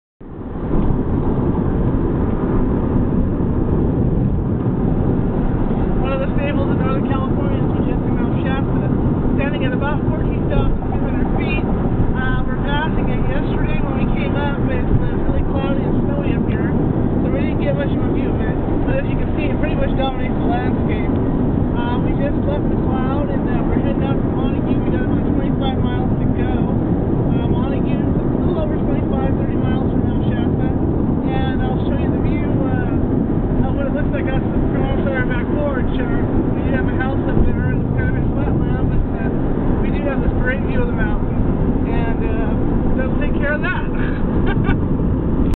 I-5 and Mount Shasta
n our way back from Greenville, we see the mountain for the first time since arriving in the area. he weather had been snowy and cooold.